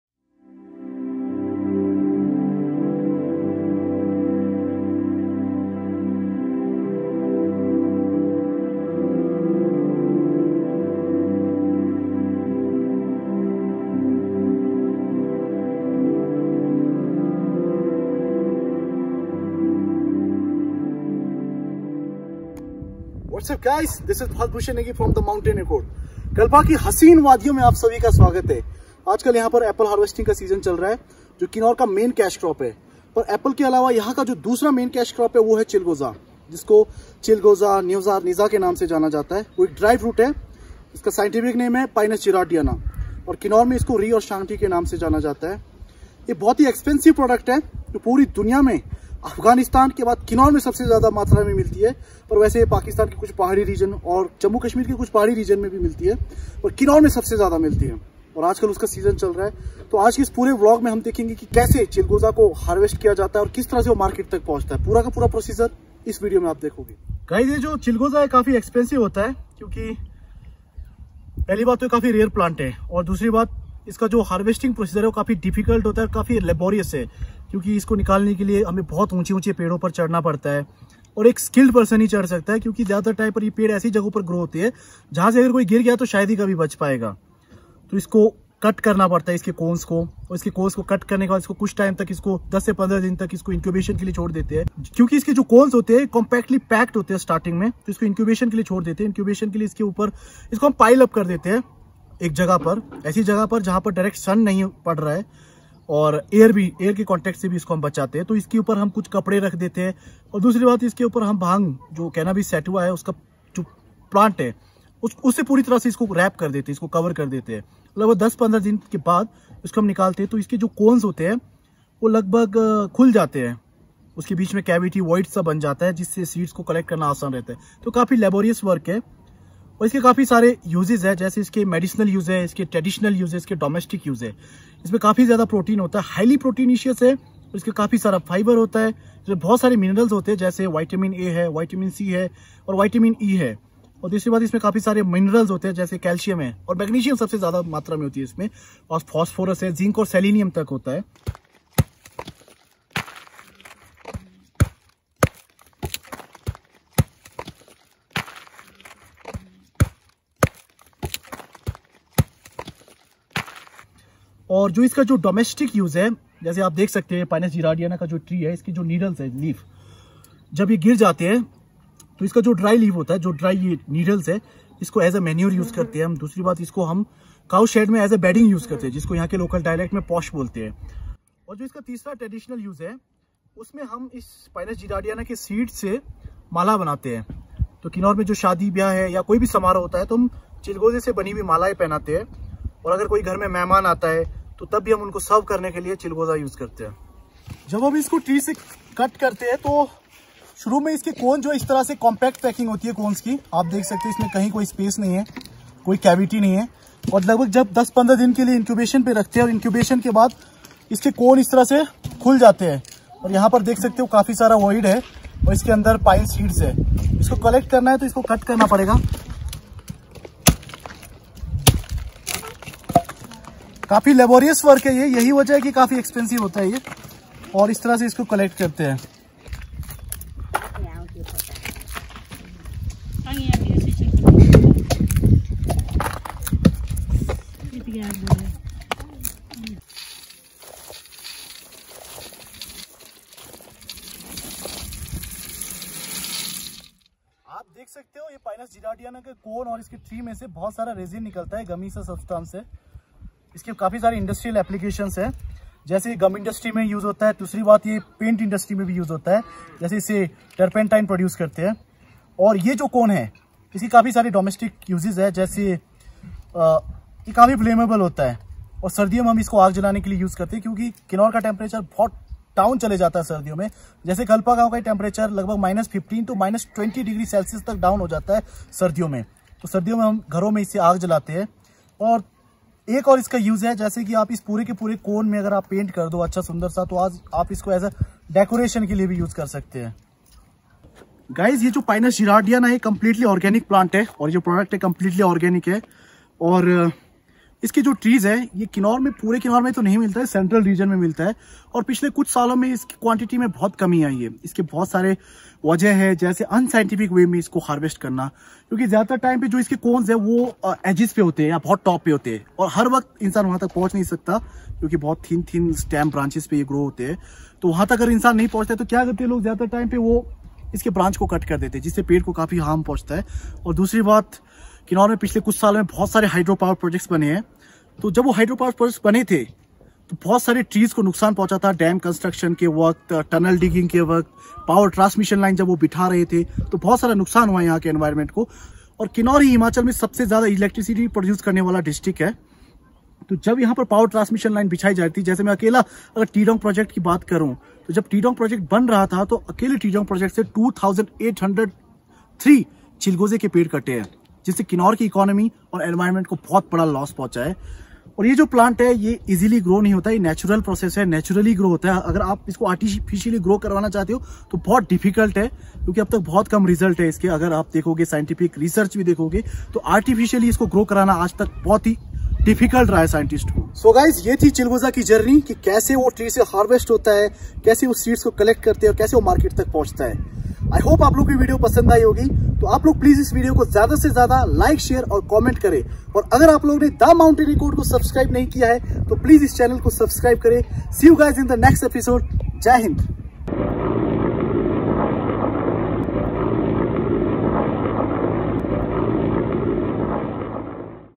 Chilgoza Harvesting || Most Expensive Dry Fruit || Kinnaur || Himachal Pradesh |Organic Fruit| INDIA
Chilgoza is one of the most expensive dry fruit which is an organic product obtained from the cones of the Chilgoza tree (Pinus gerardiana) also known as the neoza or neja. Chilgoza is a pine native to the northwestern Himalayas in eastern Afghanistan, Pakistan, and northwest India. In India, it is found in Kinnaur district of Himachal Pradesh state. It is known as Ree or Shangti in kinnauri dialect which is harvested in the month of September-October. It is a rich source of proteins and unsaturated fatty acids, it also contains Vitamin-A, Vitamin-C, and Vitamin-E along with iron, zinc, and magnesium.

Created By: Bharat Bhushan Negi
Camera: Nishant Negi (Monu) and Akshay Negi
Edit: Bharat Bhushan Negi